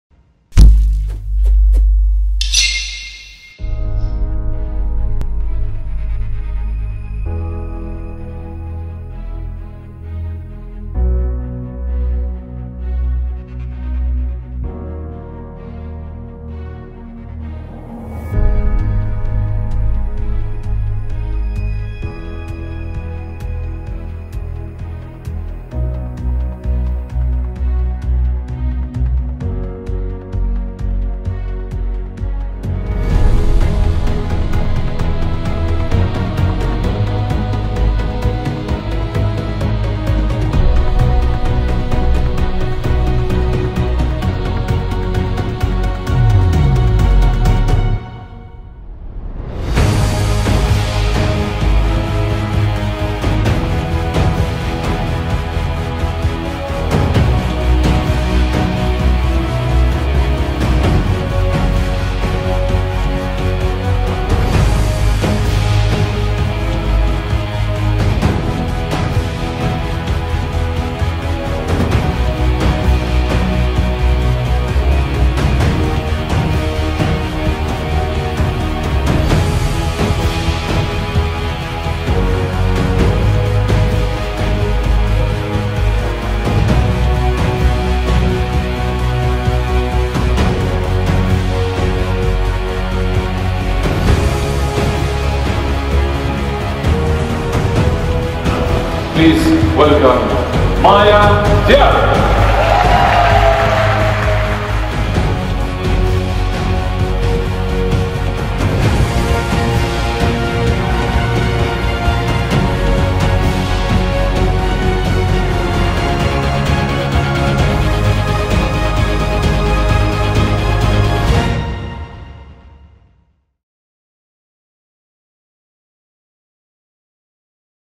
Samsung Launching of the Note 8 by Kristie's
Location: Forum de Beyrouth
Date: 28th of September 2017
Number of invitees: 600p.